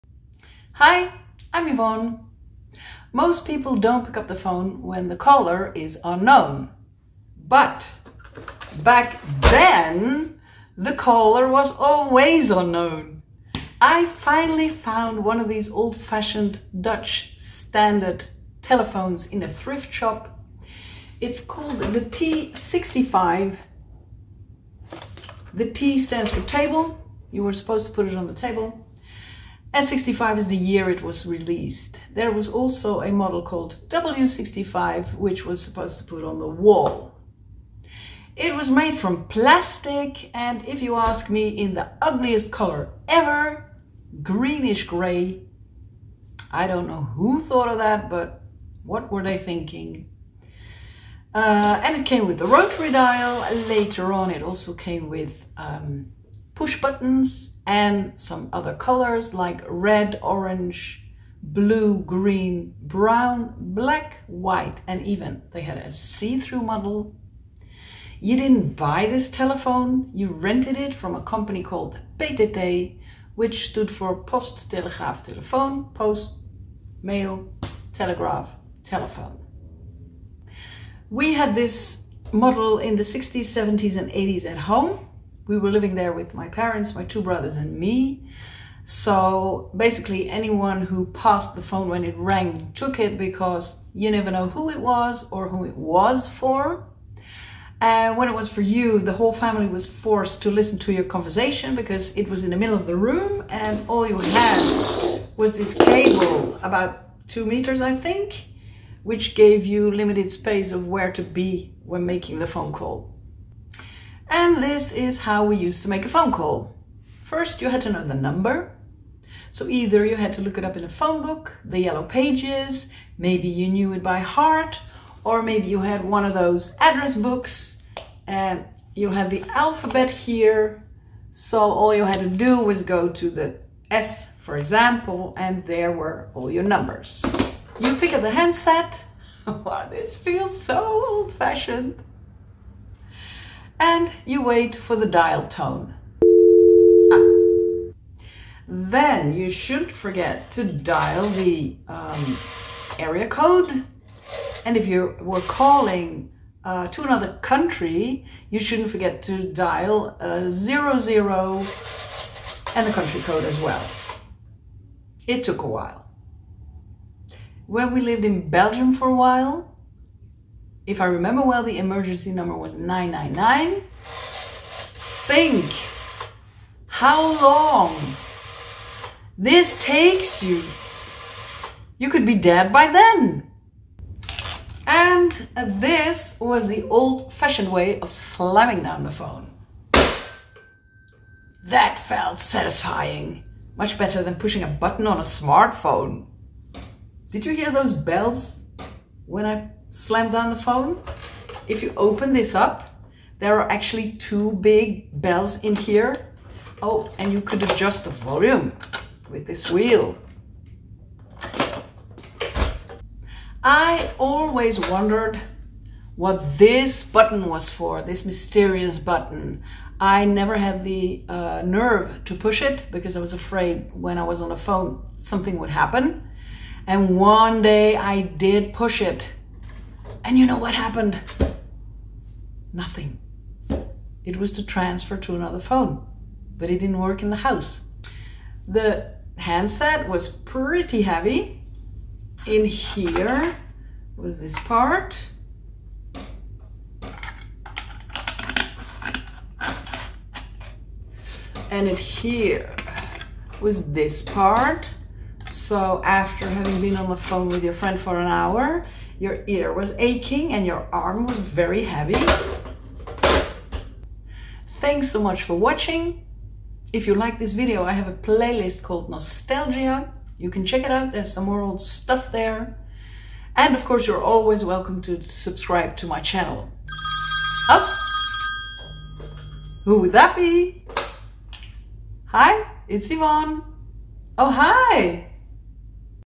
Using an old-fashioned rotary dial telephone
What used to be a dull everyday household object, has now become a cool vintage must-have! Here is how the old-fashioned Dutch rotary dial telephone, called T65, was used.

TIMESTAMPS
00:00 intro
00:13 introducing the vintage telephone
01:20 when the phone rang
01:54 how to make a phonecall
02:47 emergency number
03:05 slamming down the phone
03:18 the bell
03:33 the white button
03:58 the handset
04:24 outro